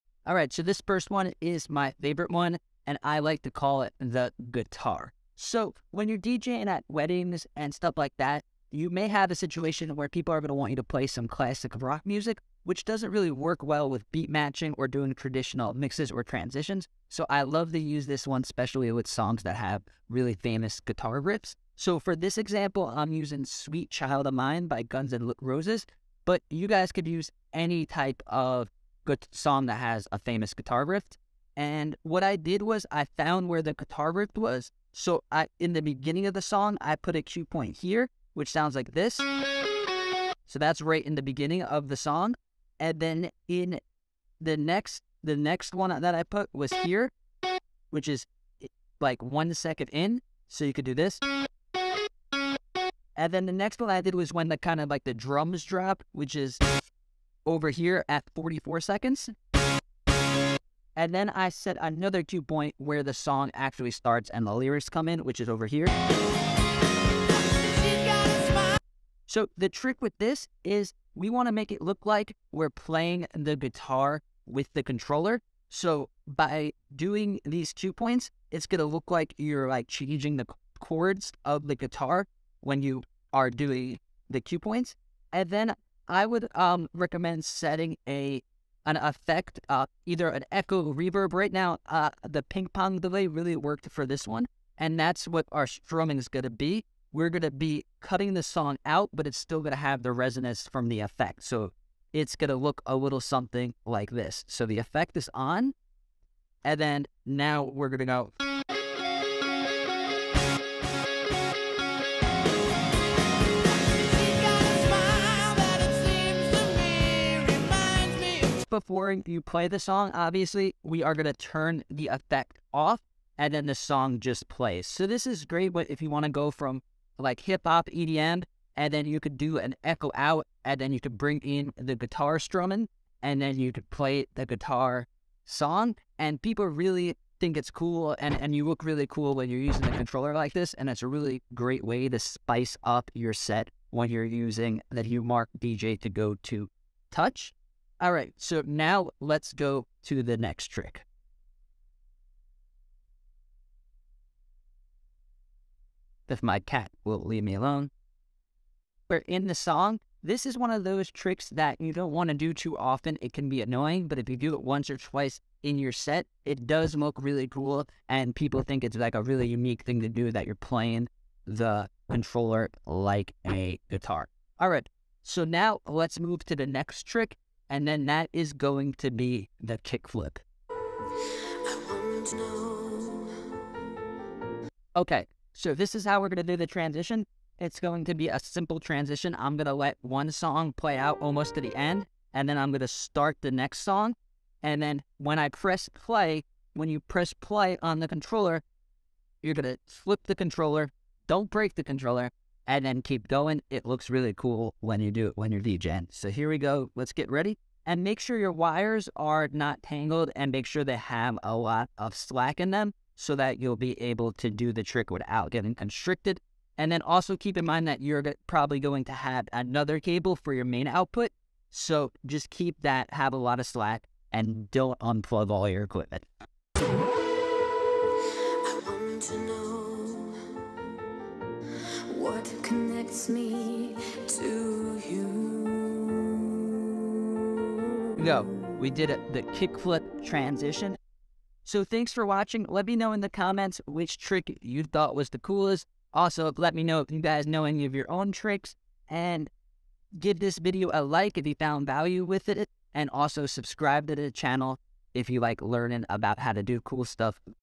Expert Tricks with the Numark DJ2GO2 Touch!
Learn some awesome tricks for using the Numark DJ2GO2 Touch with Djay Pro AI on the iPad! This video will show you all the essential techniques

► Reloop Buddy DJ Controller

► Launchpad X Grid Controller